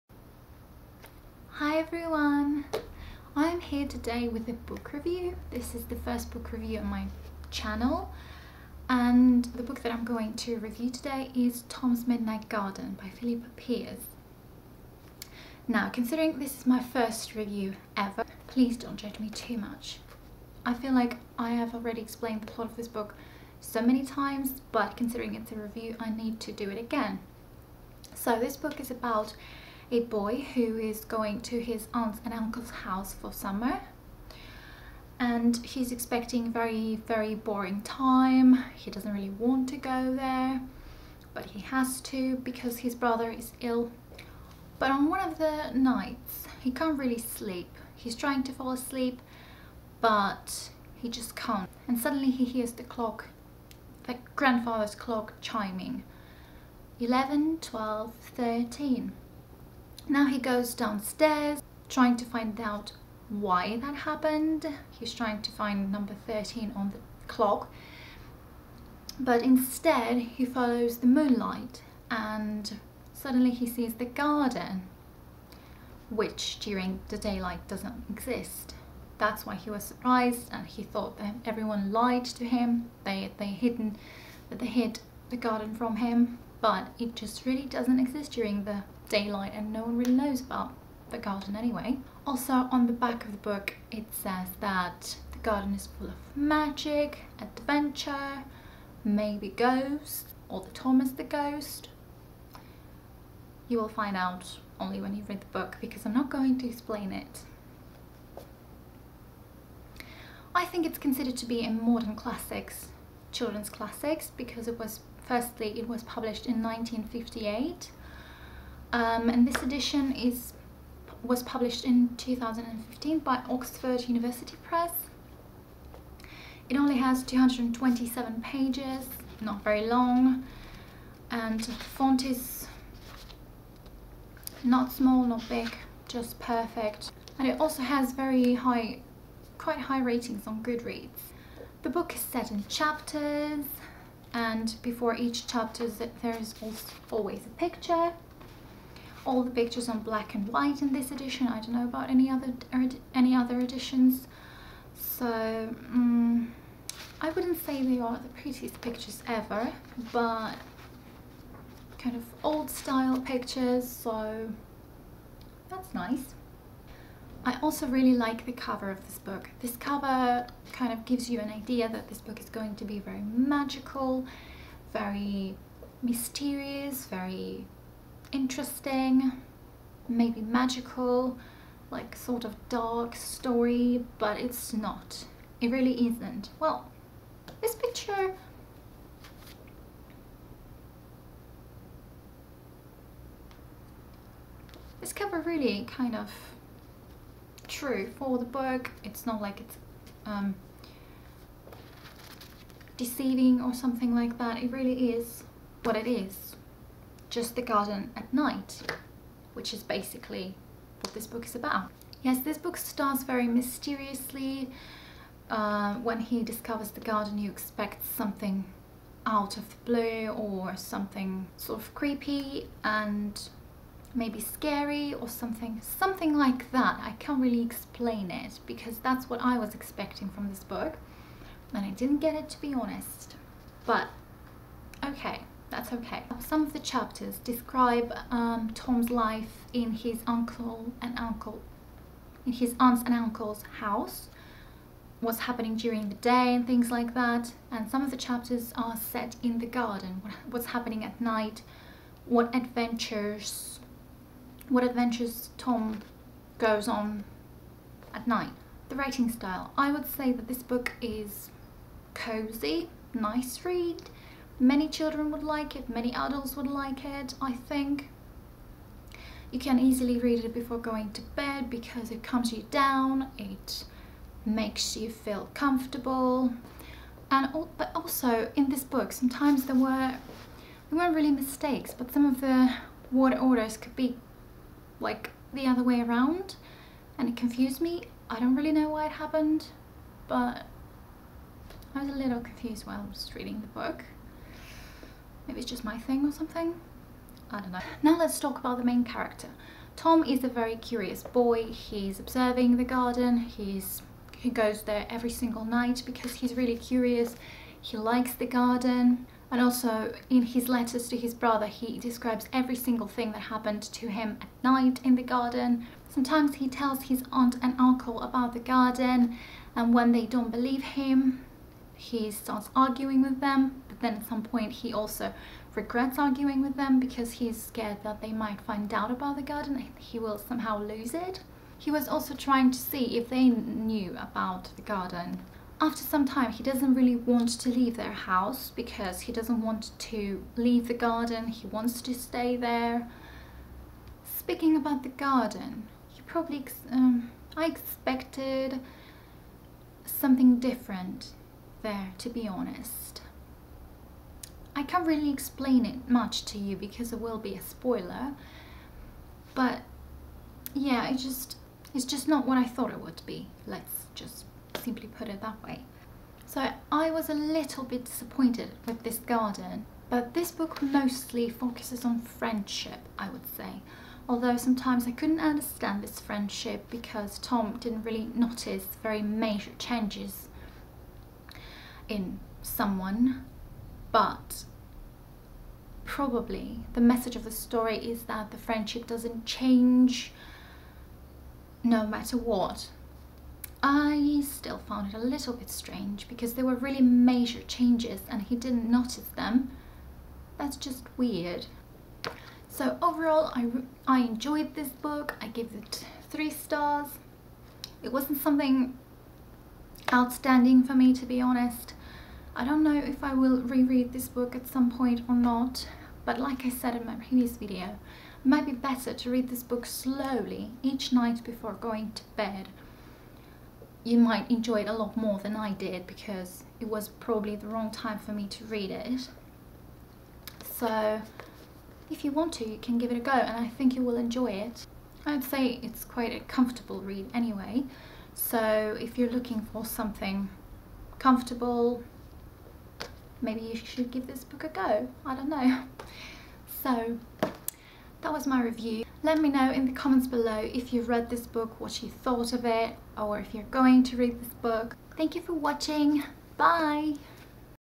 Tom's Midnight Garden by Philippa Pearce/ SPOILER FREE REVIEW
This is my first attempt to do a review, so please don't judge too much :)